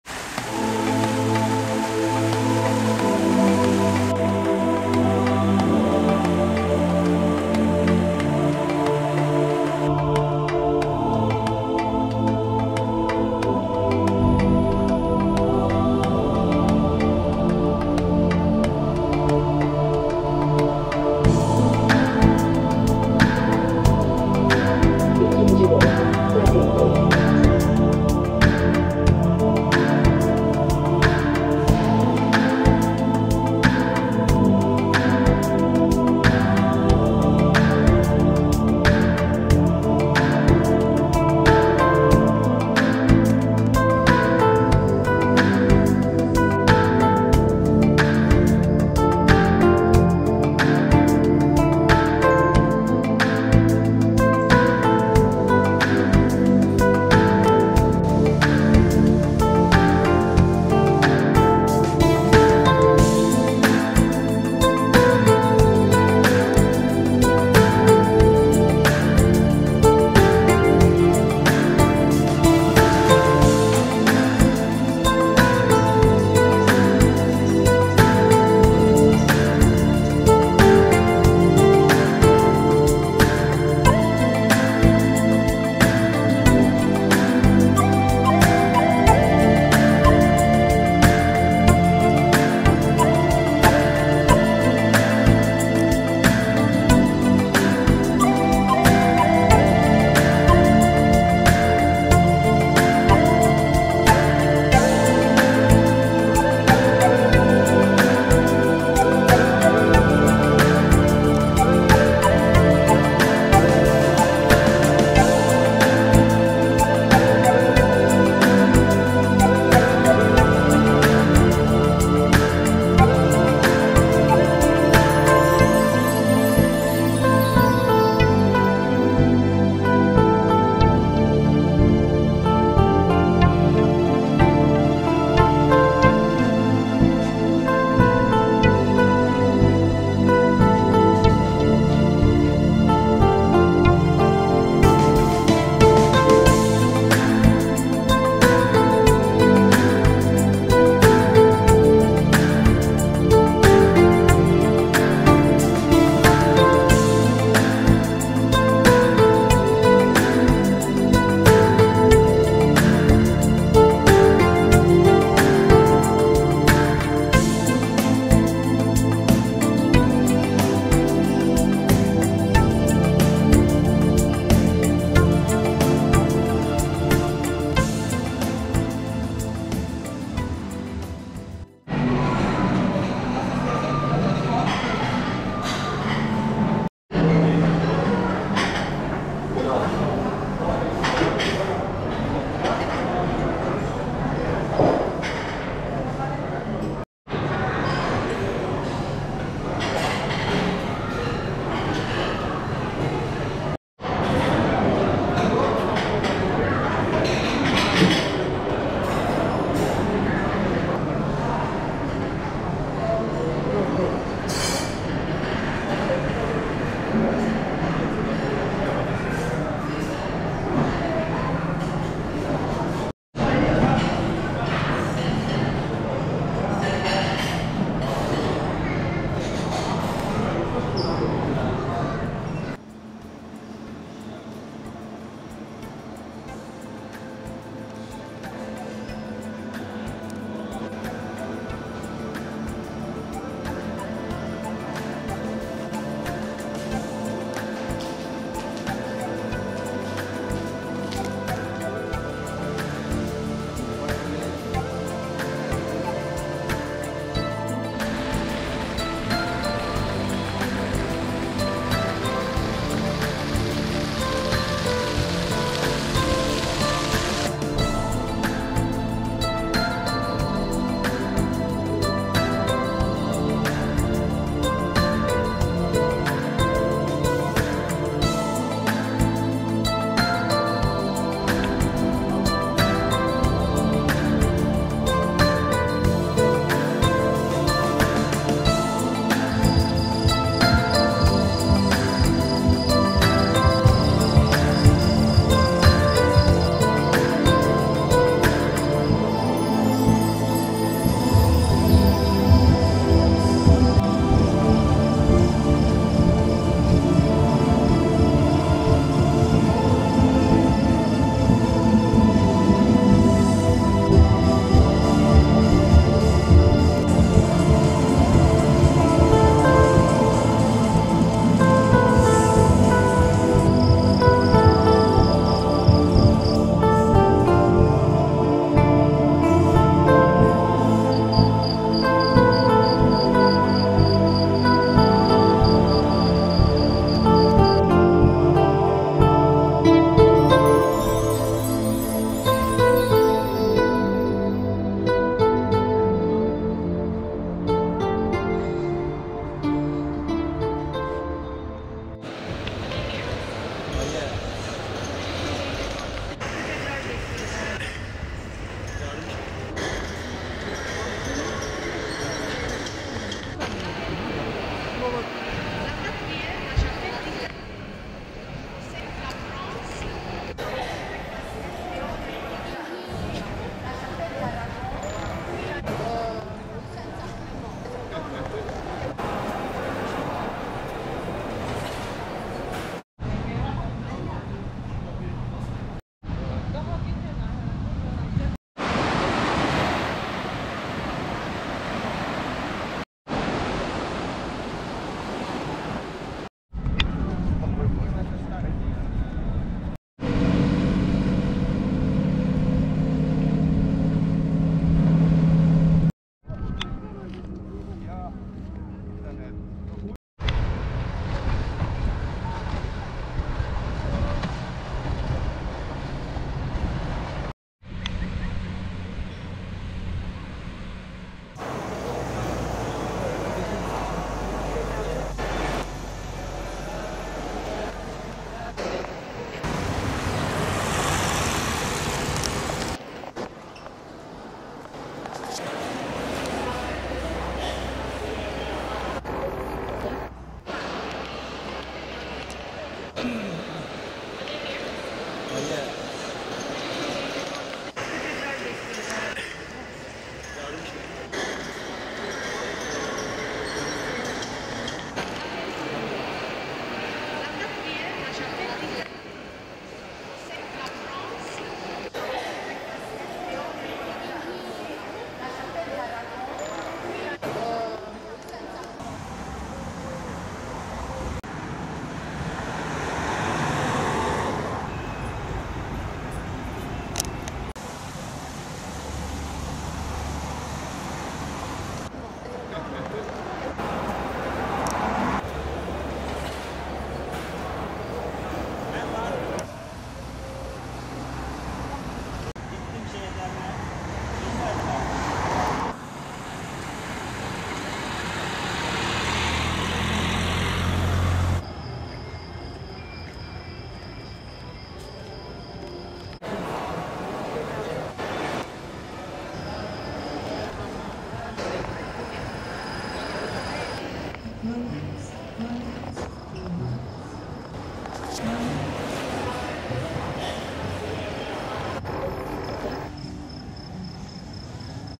Adalya Artside***** @irenetravel2366 SIDE TÜRKEI
Die Fahrt vom Flughafen dauert ca 1Stunde.Das Essen im Hotel war gut.
Ich kann dieses Hotel einfach nur weiter empfehlen.